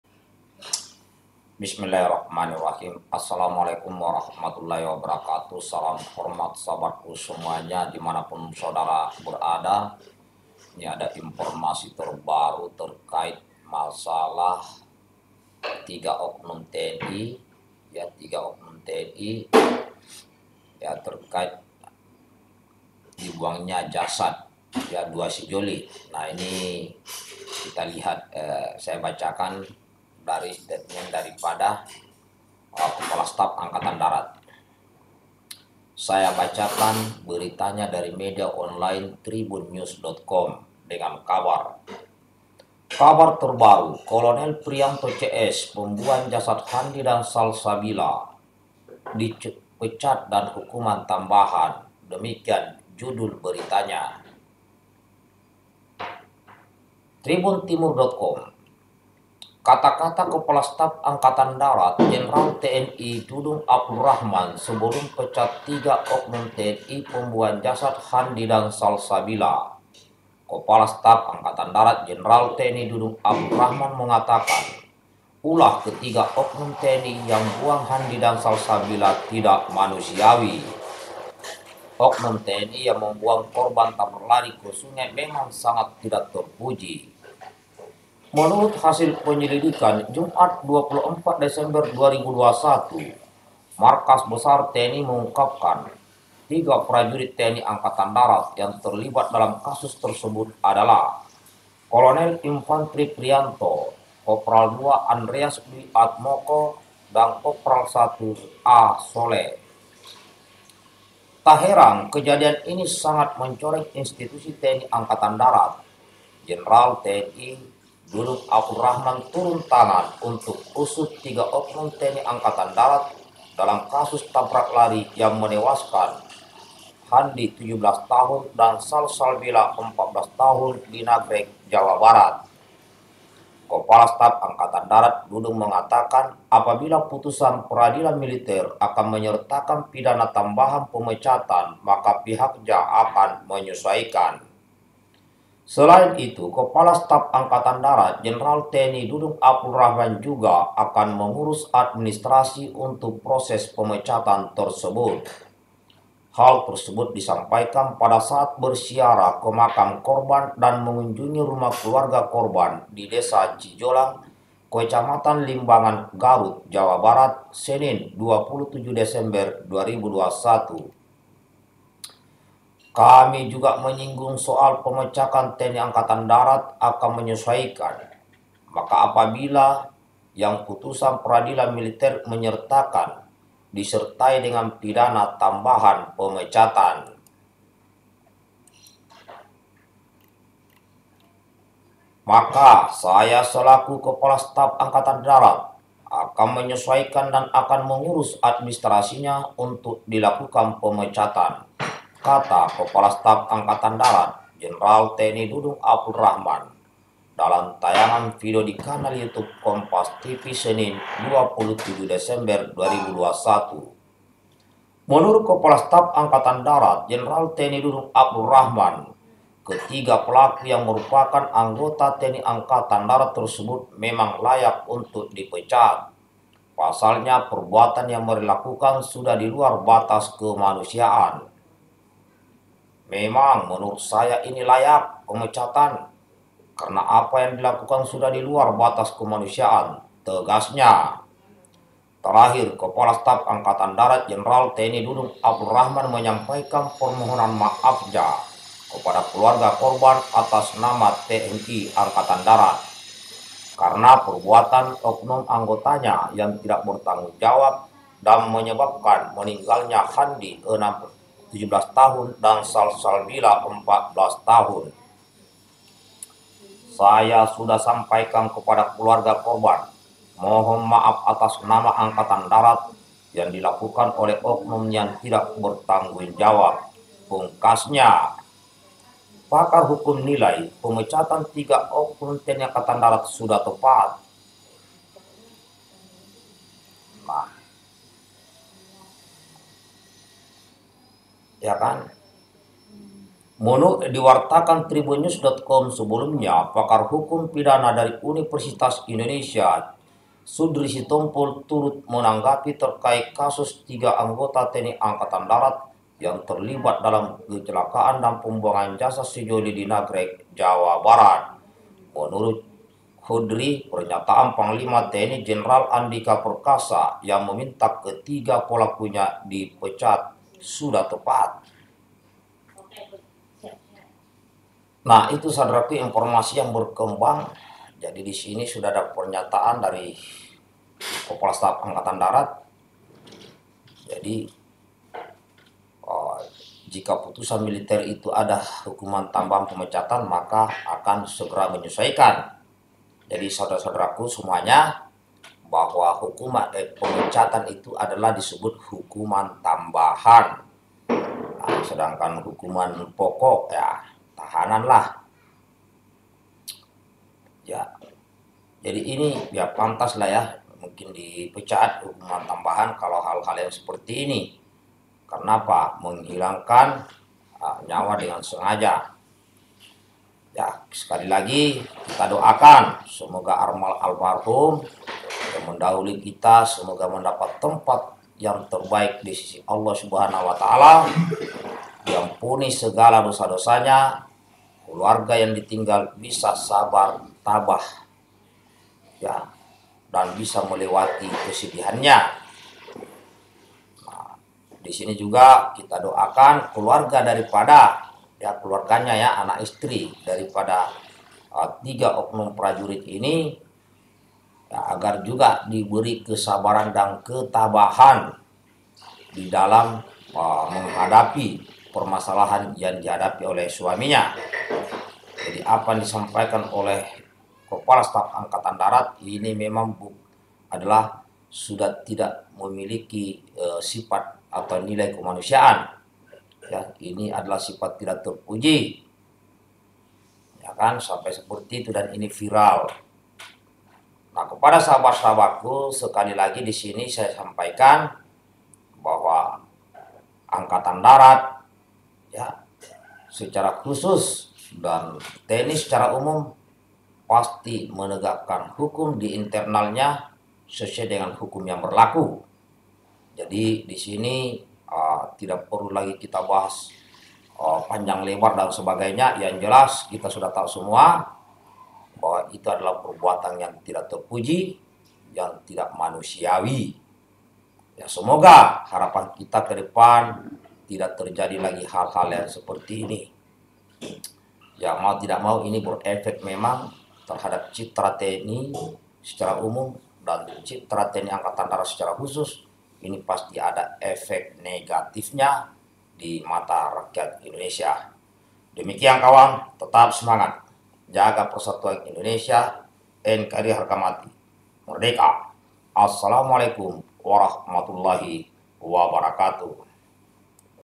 KASAD JENDERAL TNI DUDUNG A Siap Pecat 3 Oknum TNI-AD Terkait Tabrak Lari. MPS : LAYAK.
Aamiin.

TTD